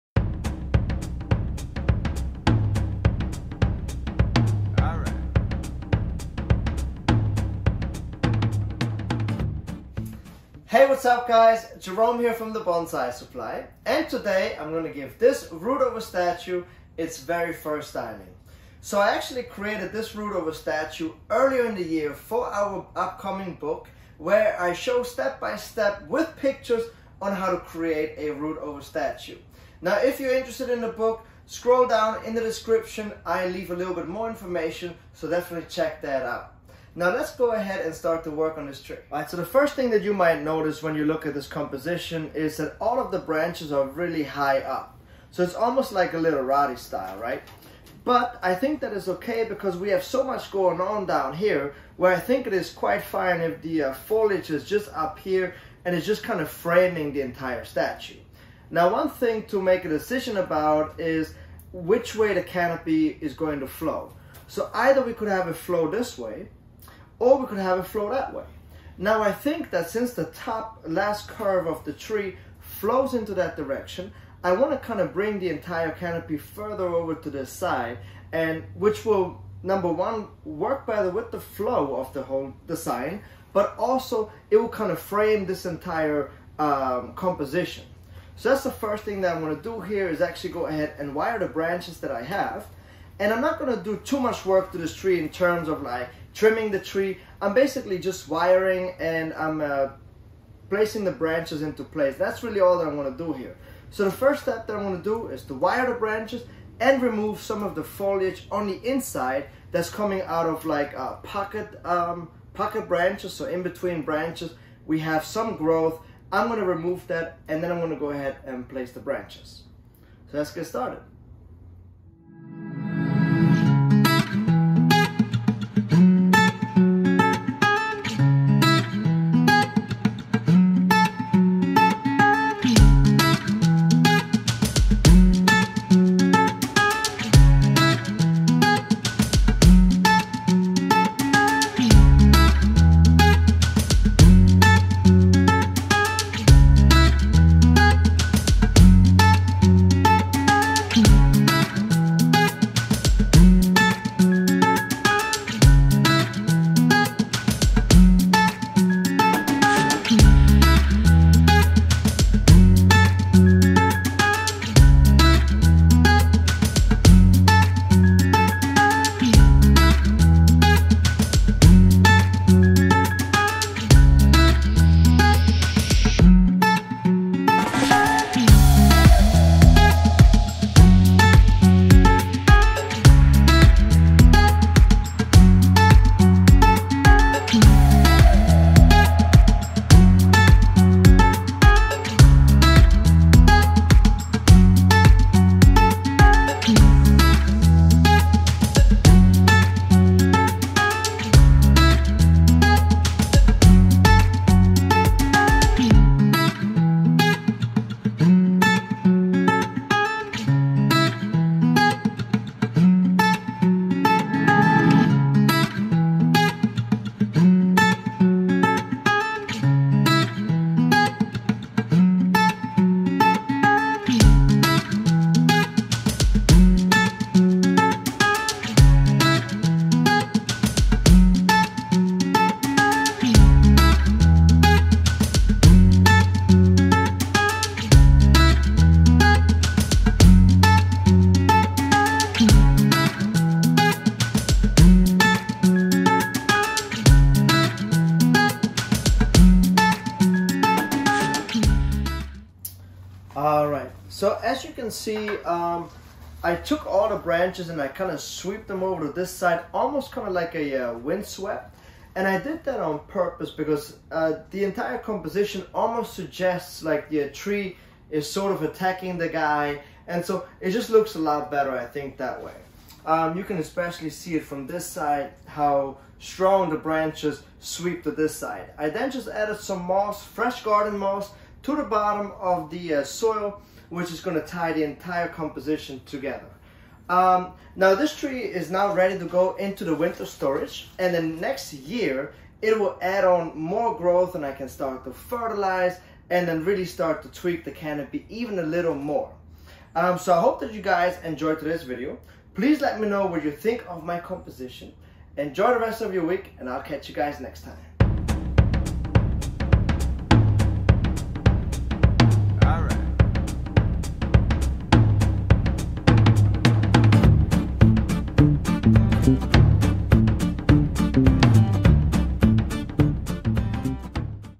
Kung-Fu Juniper (Root over Statue) Bonsai Makeover | The Bonsai Supply
Let's give this recently created Root Over Statue we called Kung-Fu Juniper, its first styling.

Looking for Bonsai Soil?

All of our amazon recommendations for other Bonsai Supplies can be found

Mari & Jerome